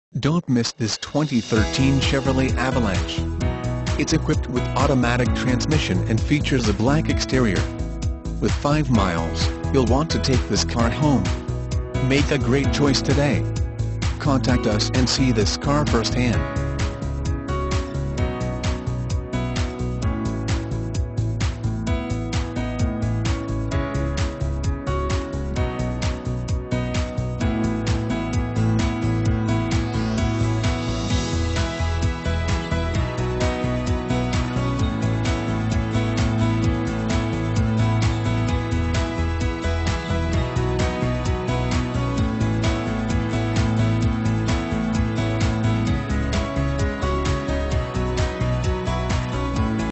2013 CHEVROLET AVALANCHE Austin, TX DG213322
2013 CHEVROLET AVALANCHE Austin, TX
Stock# DG213322

Henna Chevrolet
8805 North IH-35

Don't miss this 2013 Chevrolet Avalanche. It's equipped with Automatic transmission and features a Black exterior. With 5 miles, you'll want to take this car home. Make a great choice today.